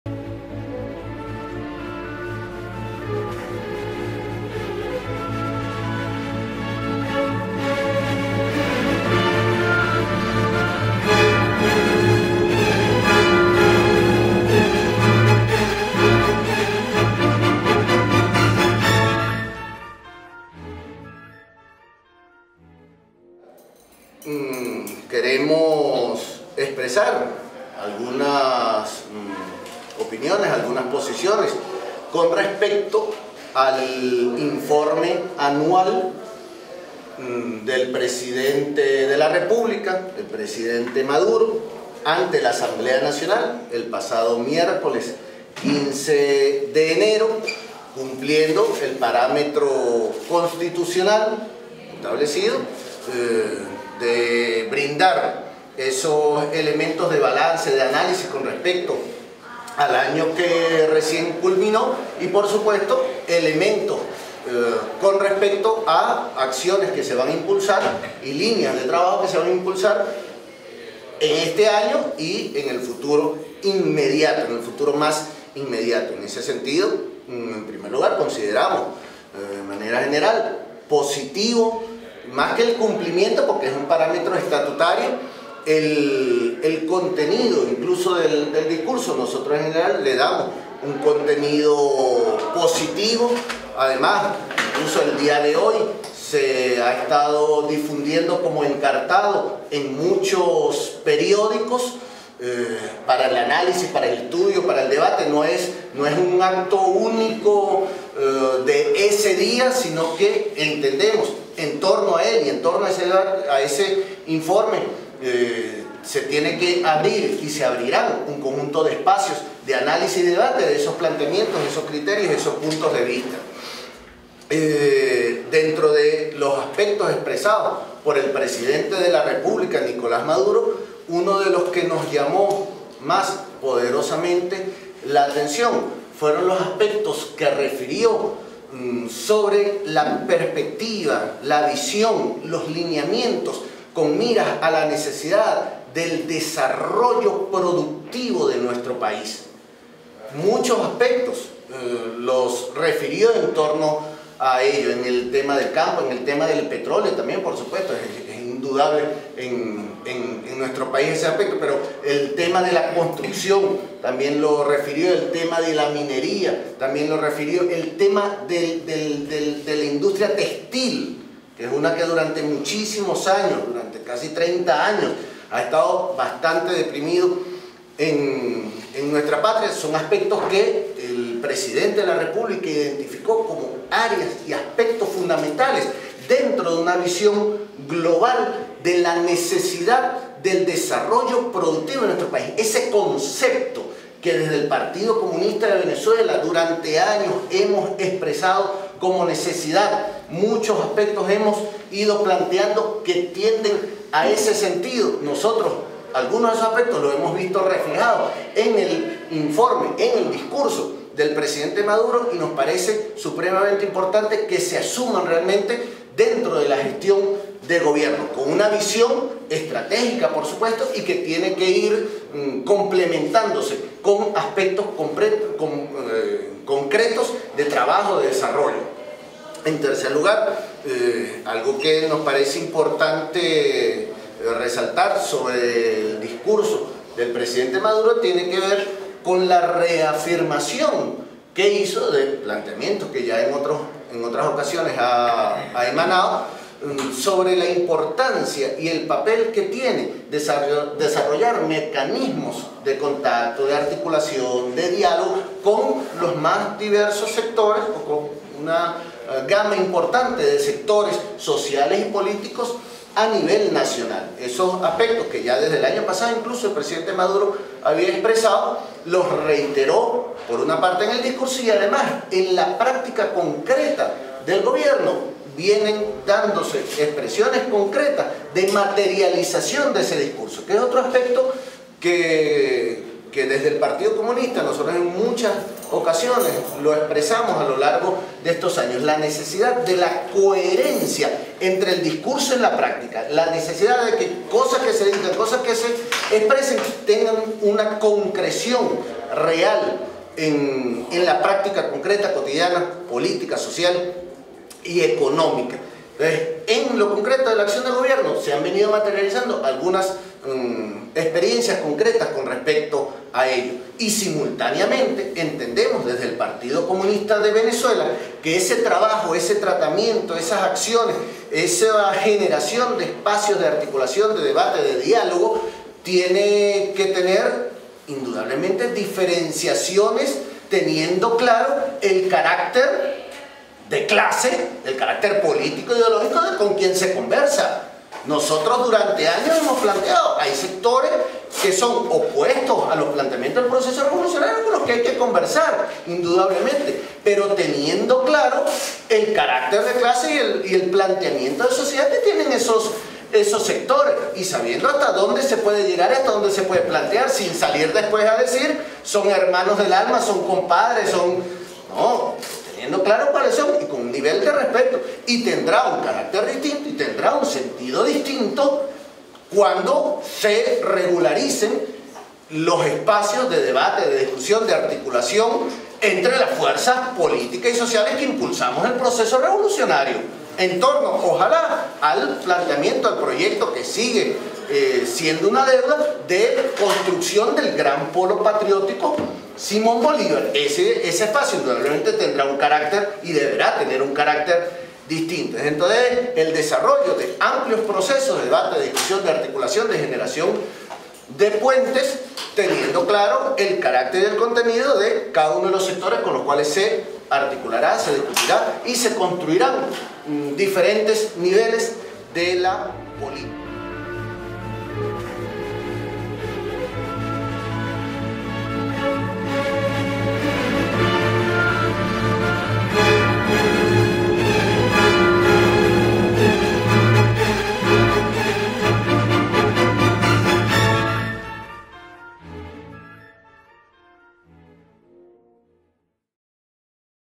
PCV llama a llevar a la práctica los planteamientos del Presidente Maduro ante la AN
Caracas, 20 ene. 2014, Tribuna Popular TP.- El Buró Político del Partido Comunista de Venezuela (PCV) analizó hoy el discurso realizado por el Presidente Nicolás Maduro en la Asamblea Nacional (AN) el pasado 15 de enero con motivo de su cuenta anual del año 2013 y pidió al Ejecutivo nacional llevar a la práctica los anuncios realizados.